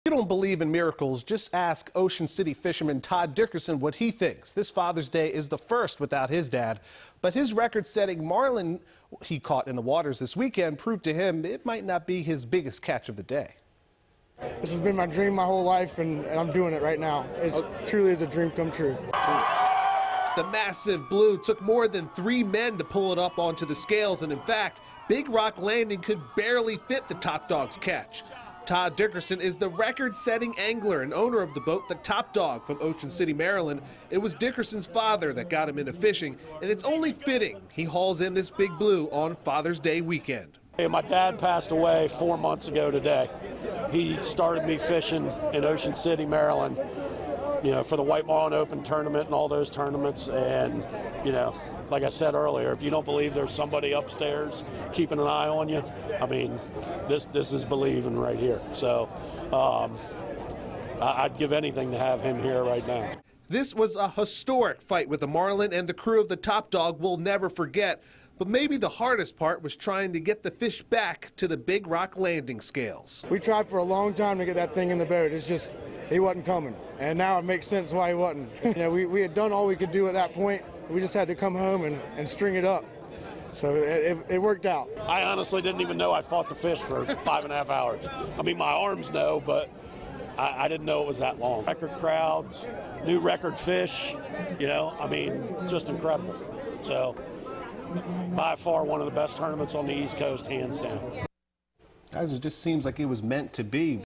Ocean City Anglers Catch Record-Breaking 914-Pound Marlin
Several Maryland anglers brought home a record-breaking catch at a fishing tournament in North Carolina this weekend.